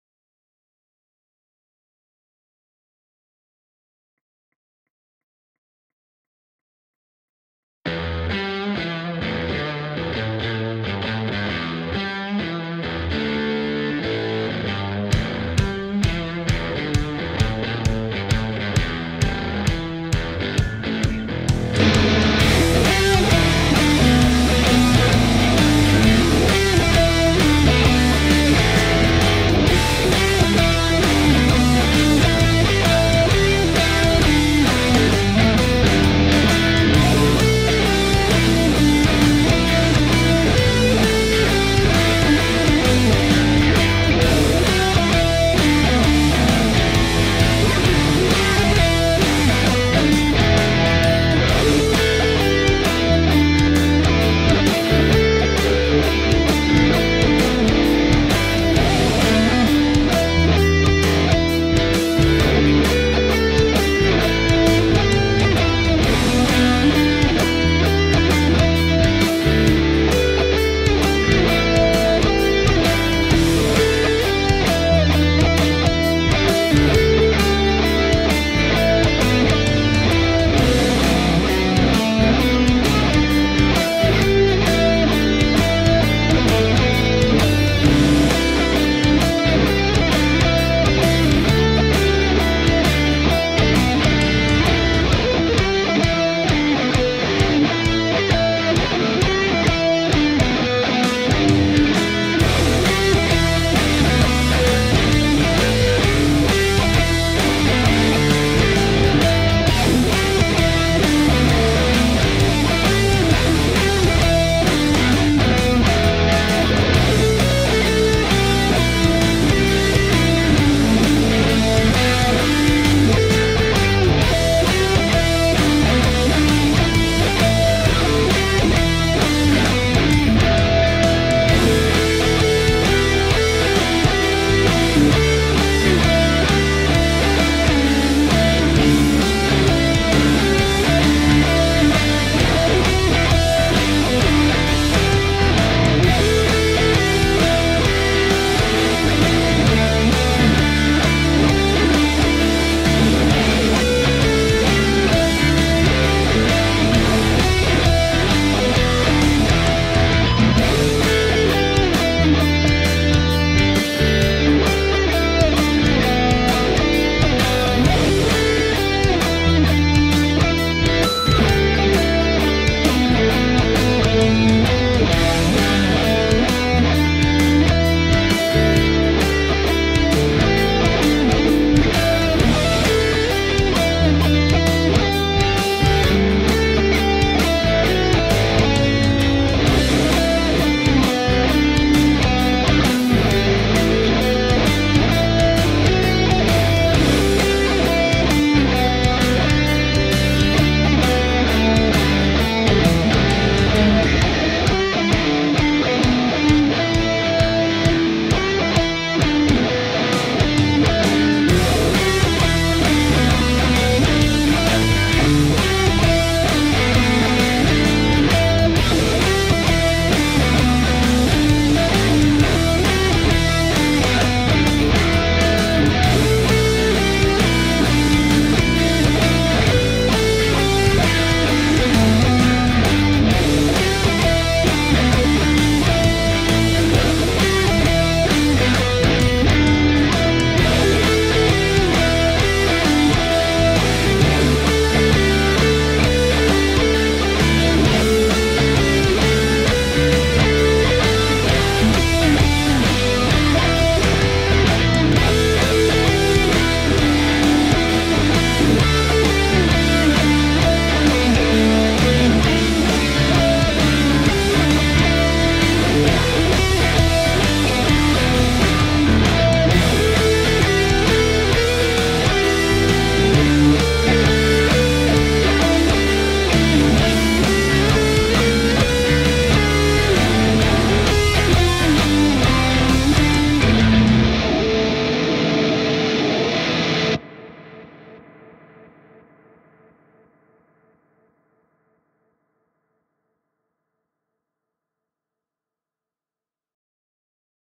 Lead over Jam track in E Dorian
Lead over Jam track in E Dorian. I like it. There are certainly BAD notes and I totally screw up the last 30 seconds.